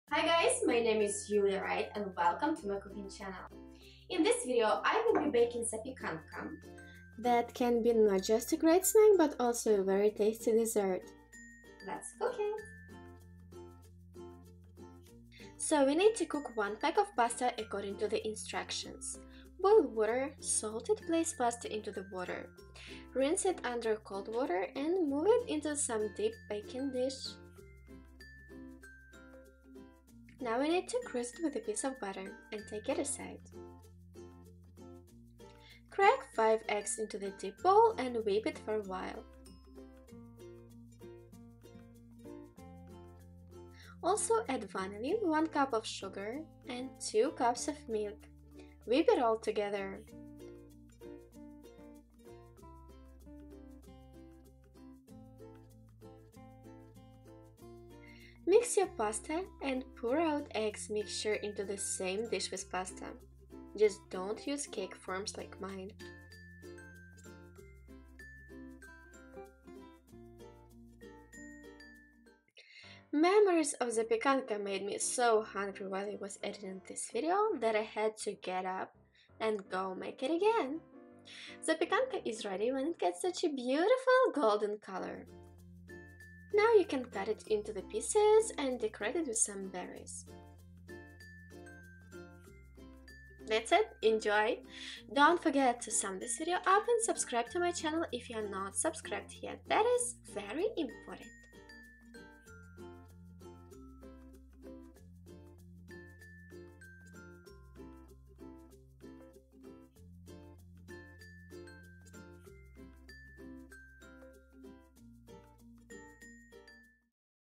Zapekanka/ Snack/ Simple cooking with Julia Ride
Zapekanka - pasta "pie". Sweet, tender, tasty snack.